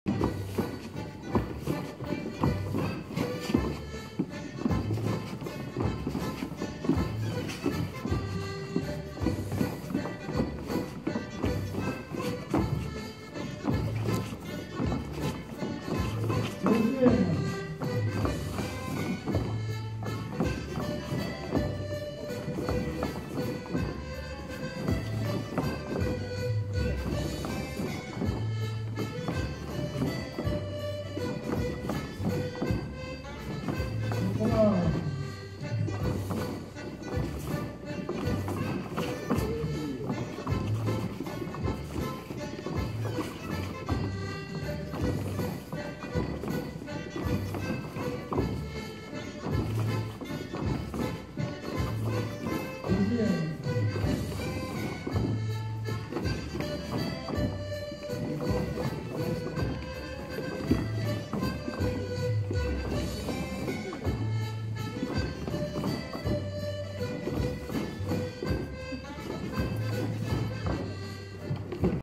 mazurka irlandaise V 2 (revisitée) dansée à Lombers 81
une autre façon plus fluide dans le déplacement, testée pour la première fois et qui semble avoir ravi nos danseurs
je la dédie à tous les danseurs qui visionnent habituellement mes vidéos...Michel